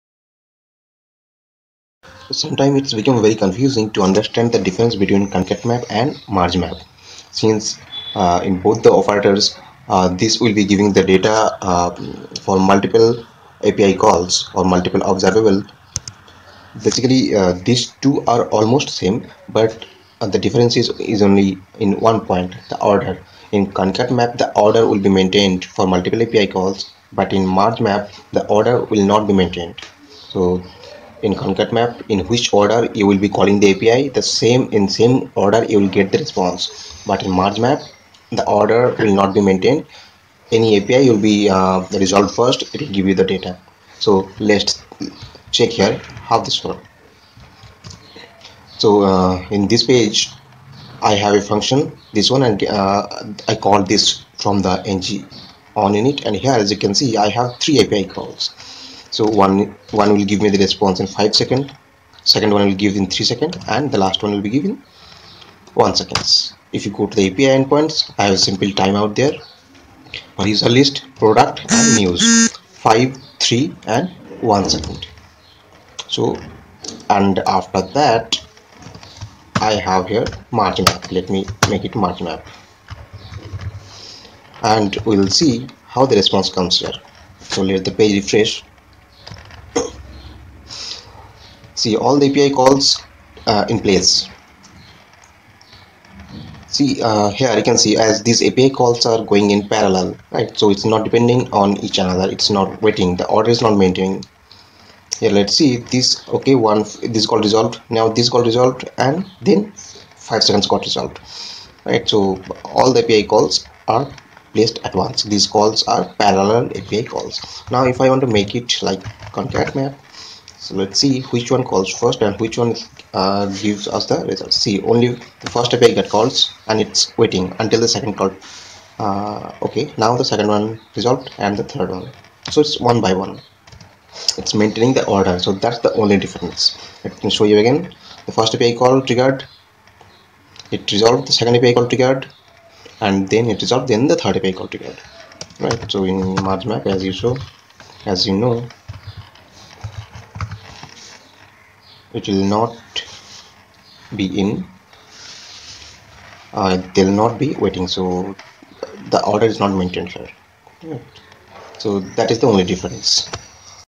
MergeMap vs ConcatMap
Here we will see the difference between MergeMap & ConcatMap with Angular example